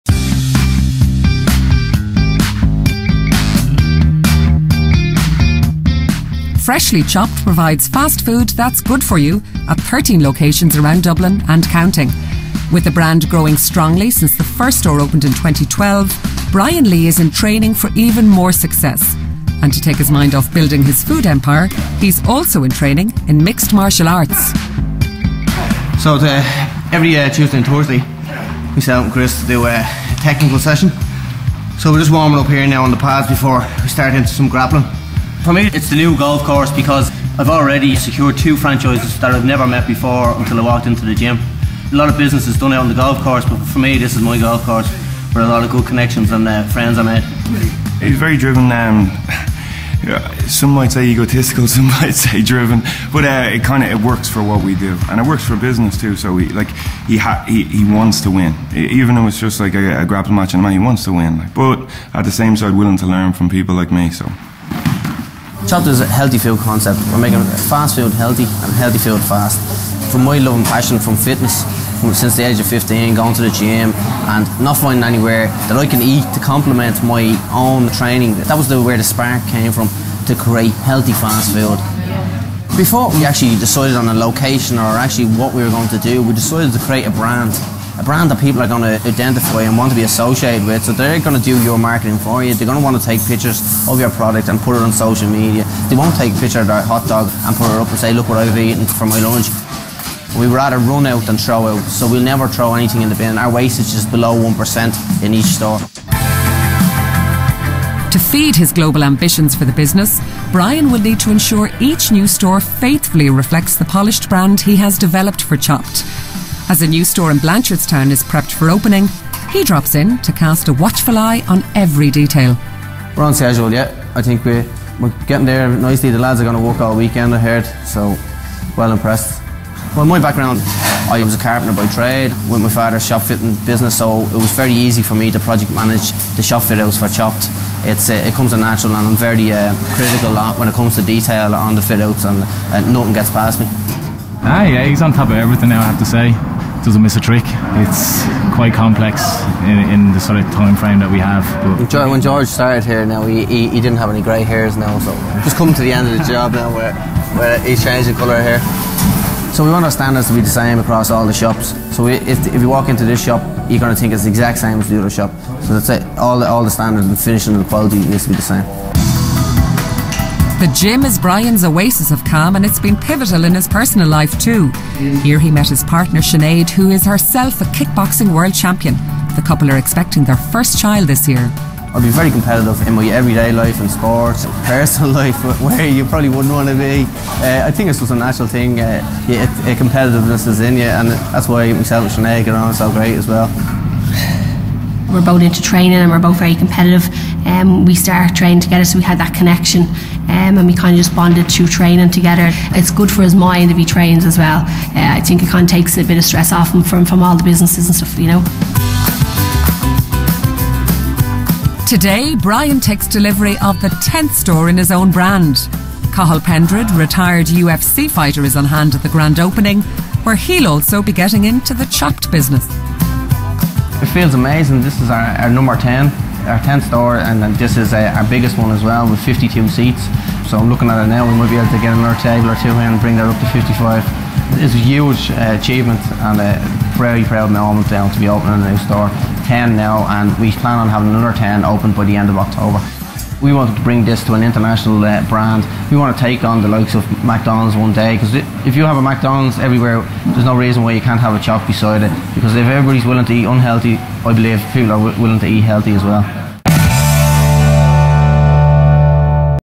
Brian Lee of Freshly Chopped - Entrepreneur of the Year 2016 finalist
We're all very proud Choppers in here today after seeing Brian on TV for EY Entrepreneur of the Year! Anyone for a game of golf?! 😂 Thanks to all who tuned in to UTV Ireland.